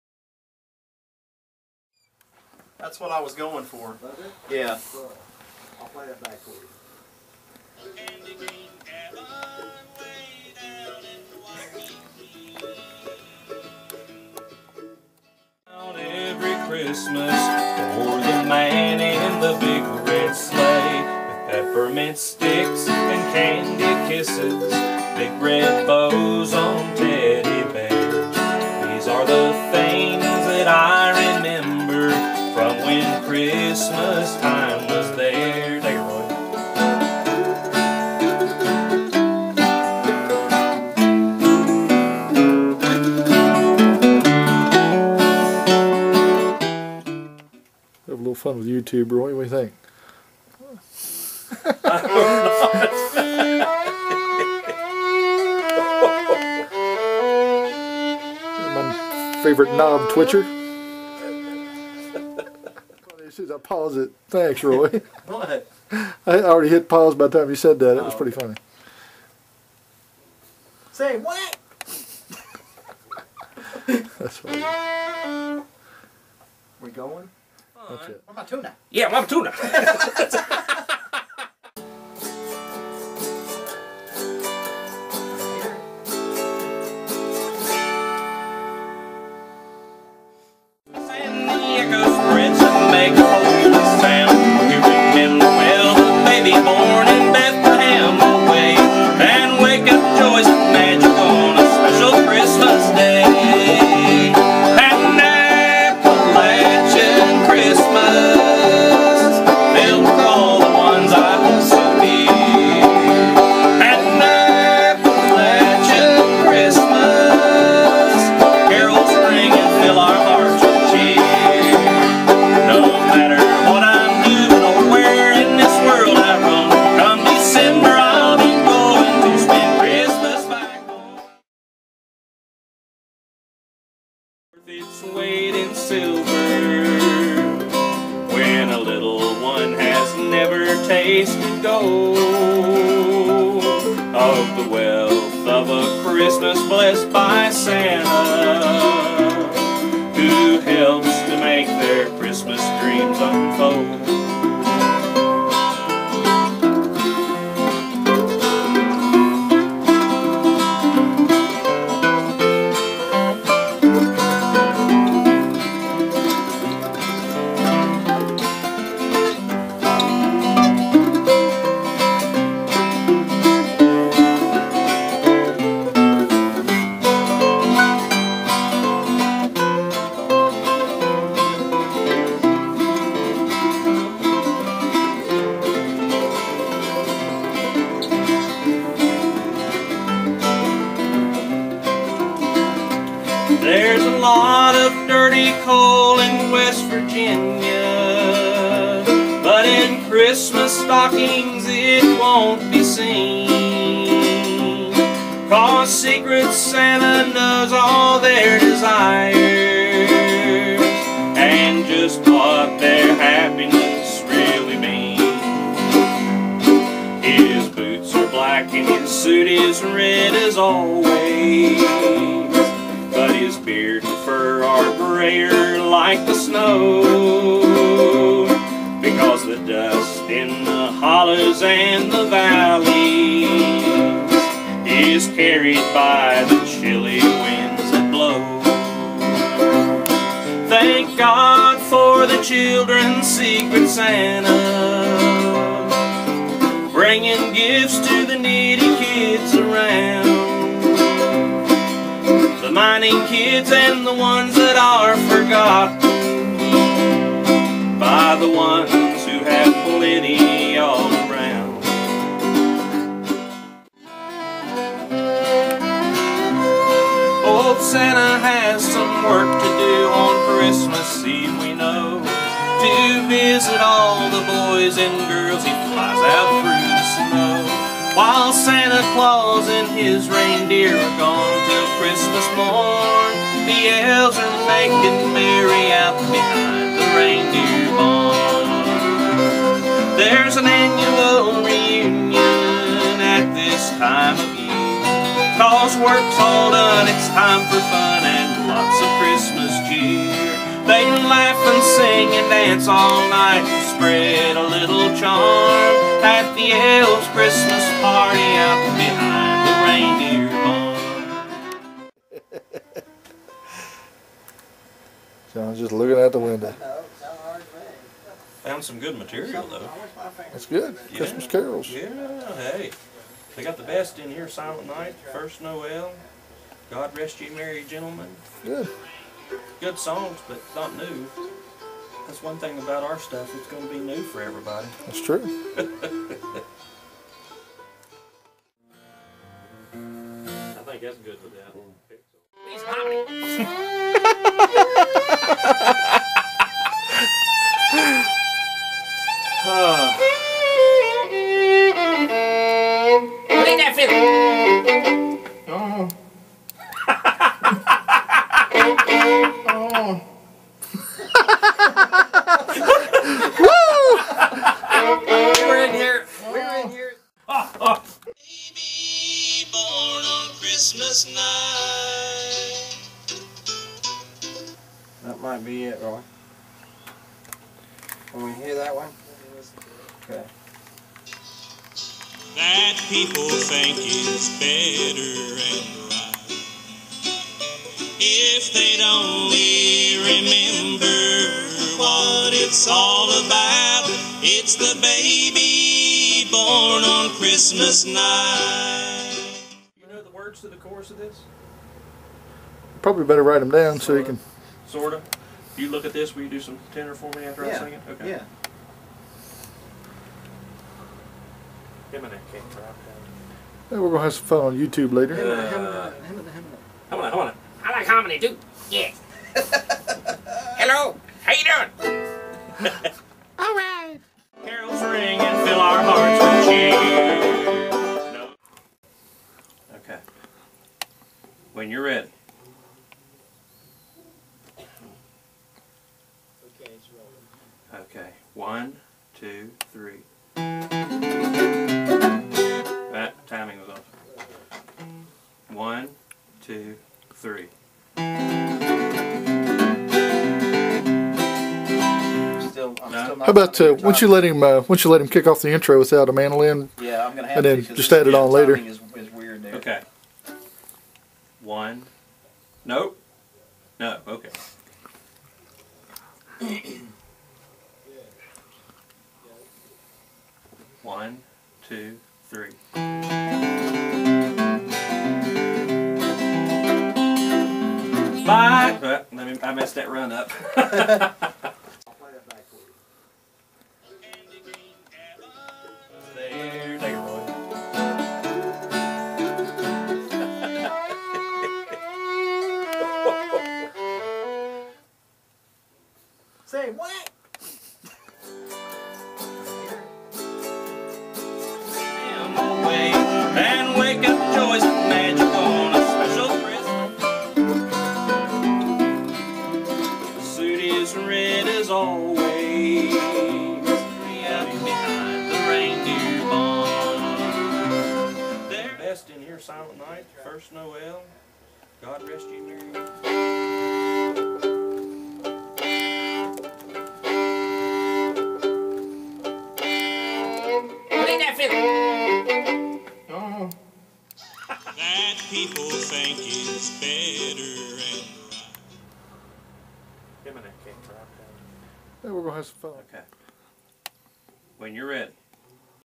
WV Secret Santa cd project
Will Price, Roy Mays, Santa John Johnson and John "Mudcat" Peters teamed up in August to record a new cd project that will benefit the WV Secret Santa organization.  All of the songs are originals and written by Will Price and John Johnson.